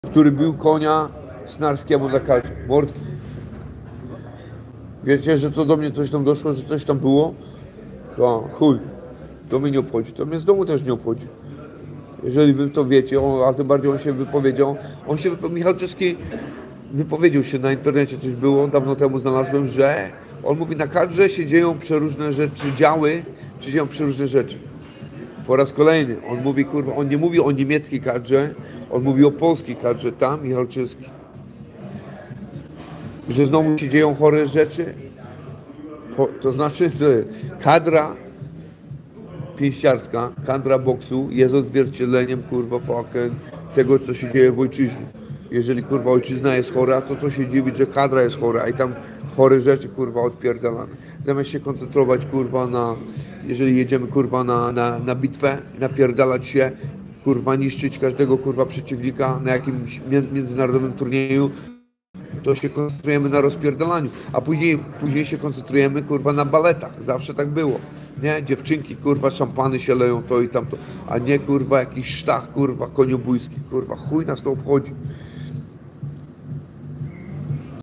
Olimpijczyk o aferze końskiej Michalczewski-Snarski
Binkowski o Michalczewskim i Snarskim, kolejna afera końska.